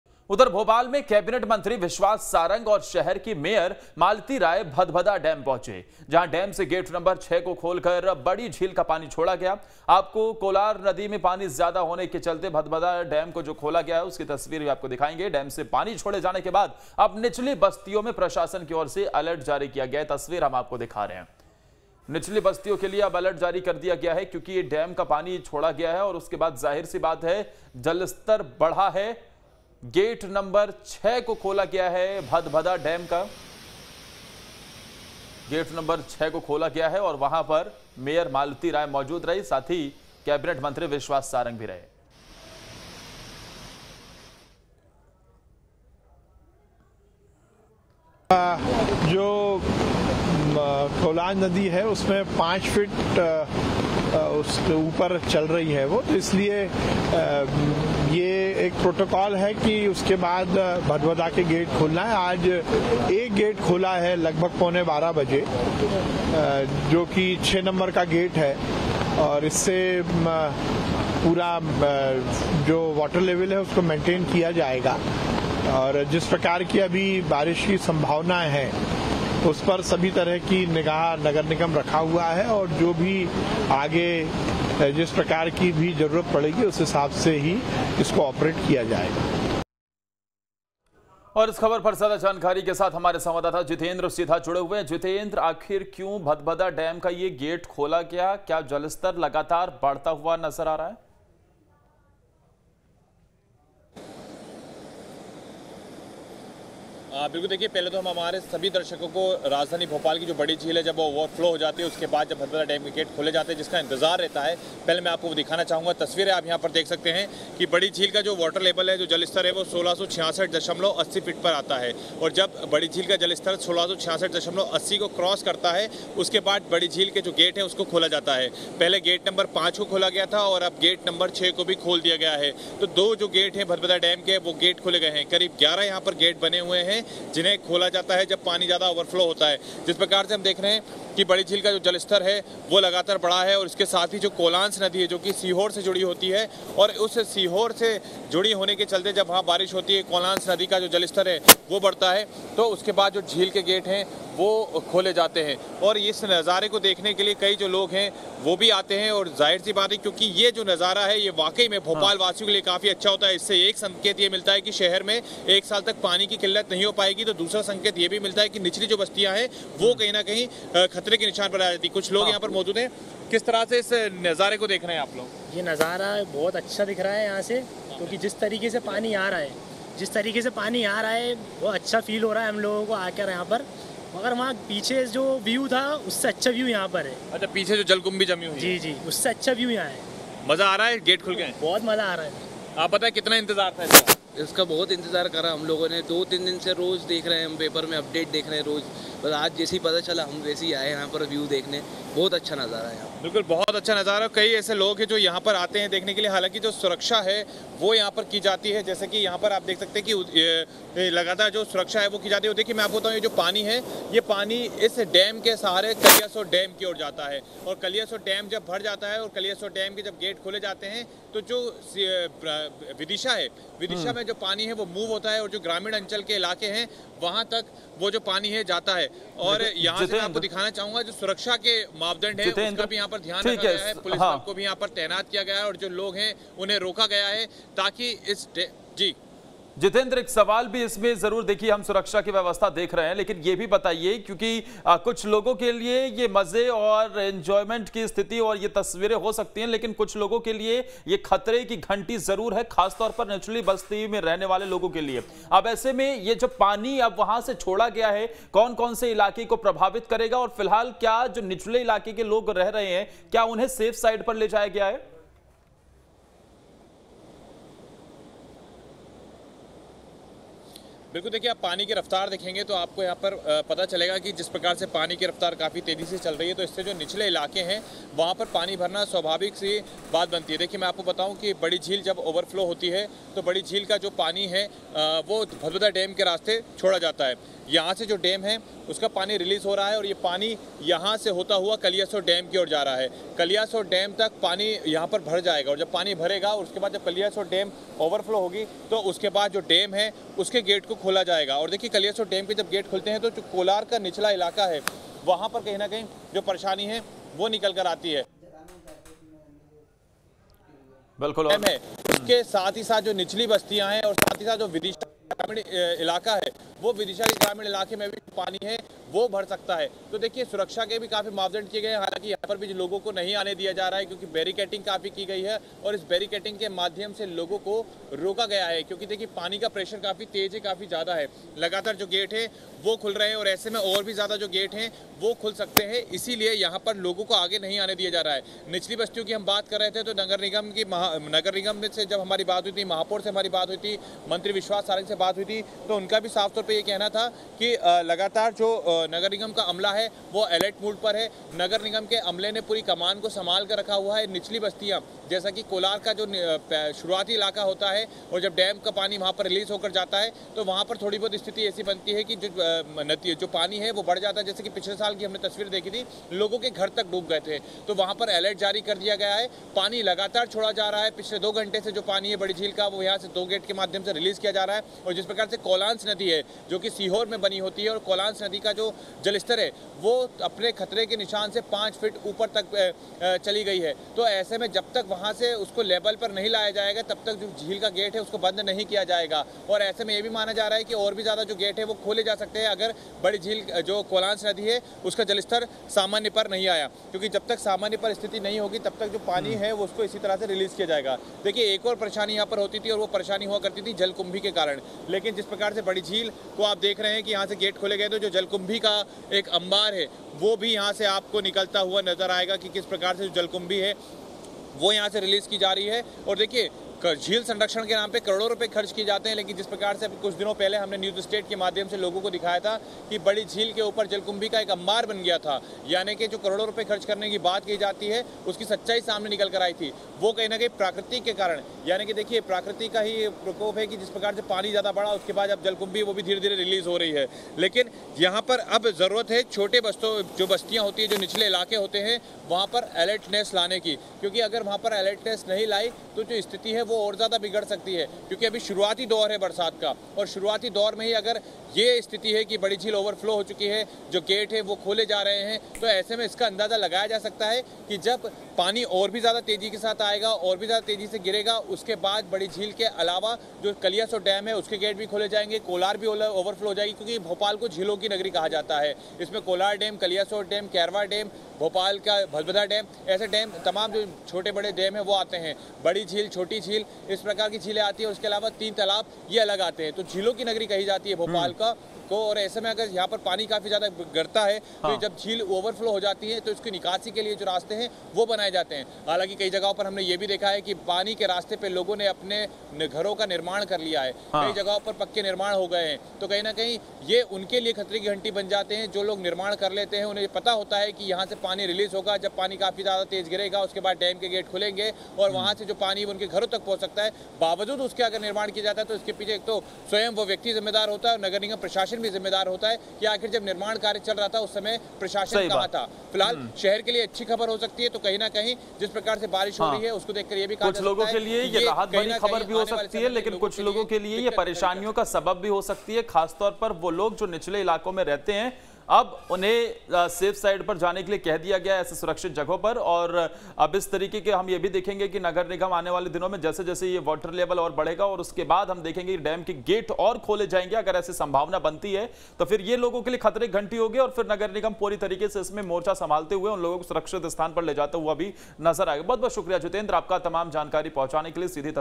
Bhadbhada Dam Bhopal: कोलार नदी का जलस्तर बढ़ने के बाद खोला गया भदभदा डैम का गेट| Bhopal Latest News
Madhya Pradesh : भोपाल (Bhopal) में हो रही लगातार बारिश के चलते बड़े तालाब का जल स्तर खतरे कि निशान से ऊपर आ गया जिसके बाद भदभदा बांध के गेट खोलने का फैसला लिया गया।